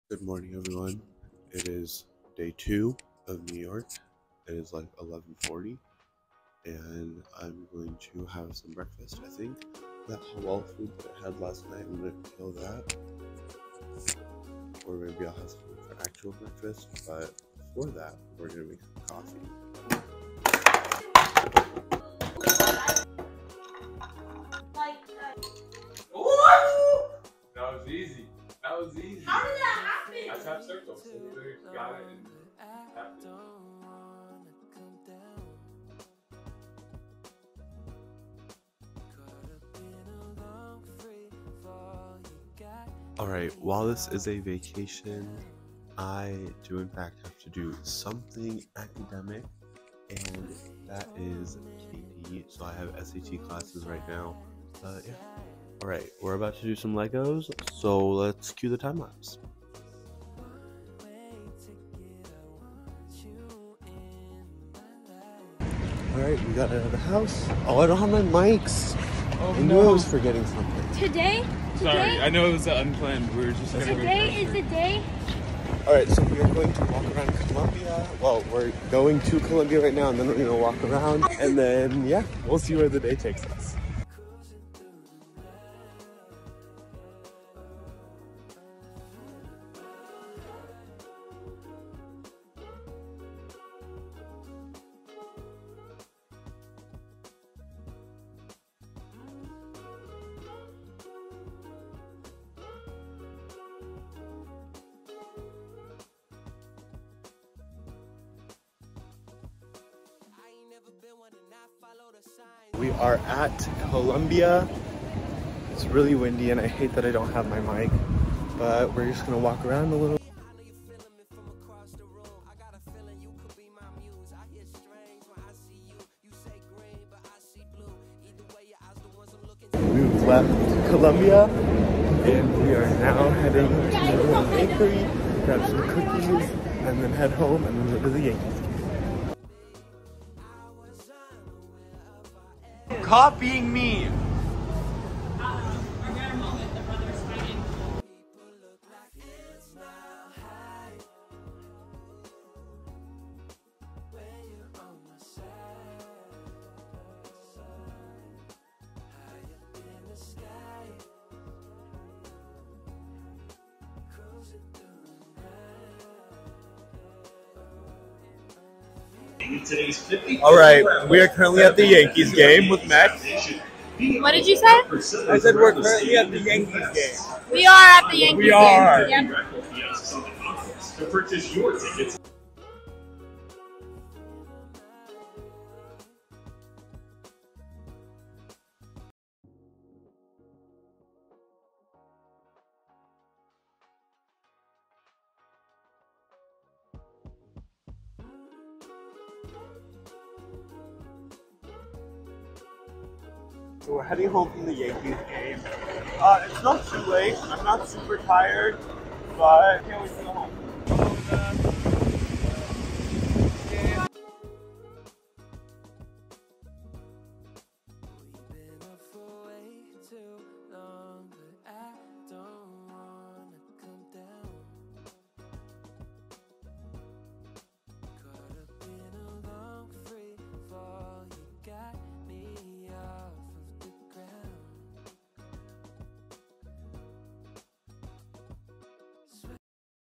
nyc day in the life! visit columbia, yankees game
Music From Thematic Music